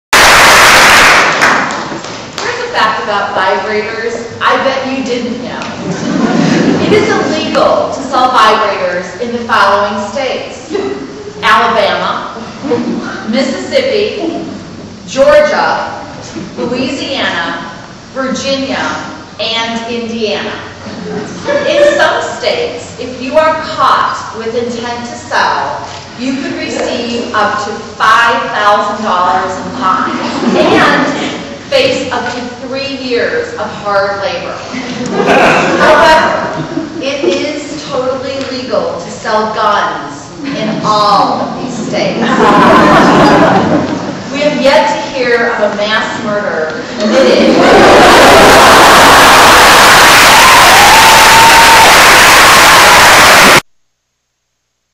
The Vagina Monologues - Vibrator Fact.
#23 Tri-Valley Haven's 2008 production of "The Vagina Monologues."

Fact - Vibrators.